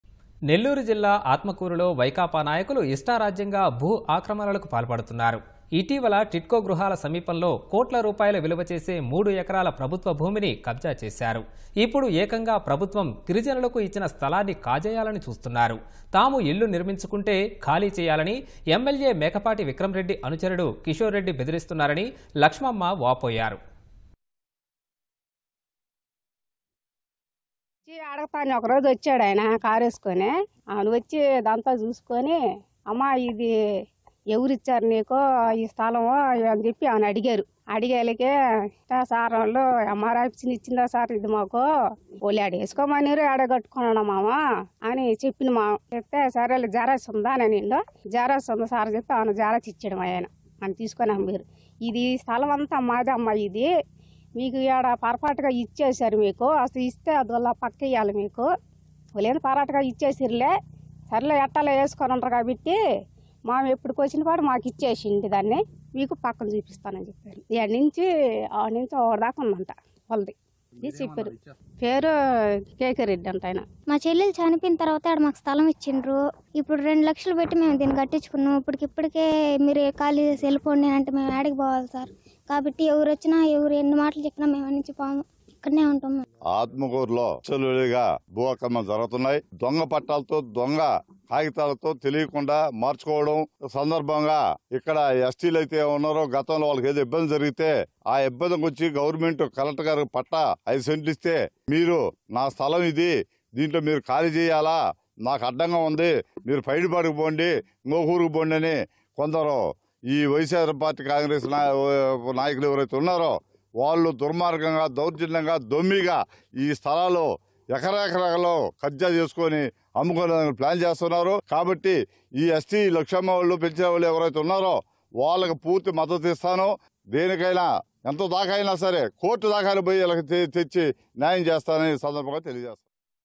వైకాపా కబ్జా కోరల్లో ప్రభుత్వ భూములు | YCP Leaders Encroached Govt. Lands Near Nellore Tidco Houses
నెల్లూరు జిల్లా ఆత్మకూరులో వైకాపా నాయకులు ఇష్టారాజ్యంగా భూ ఆక్రమణలకు పాల్పడుతున్నారు. ఇటీవల టిడ్కో గృహాల సమీపంలో కోట్ల రూపాయల విలువచేసే మూడు ఎకరాల ప్రభుత్వ భూమిని కబ్జా చేశారు. ఇప్పుడు ఏకంగా ప్రభుత్వం గిరిజనులకు ఇచ్చిన స్థలాన్ని కాజేయాలని చూస్తున్నారు. తాము ఇళ్లు నిర్మించుకుంటే ఖాళీ చేయాలని ఎమ్మెల్యే మేకపాటి విక్రమ్ రెడ్డి అనుచరుడు కిషోర్ రెడ్డి బెదిరిస్తున్నారని లక్ష్మమ్మ వాపోయారు.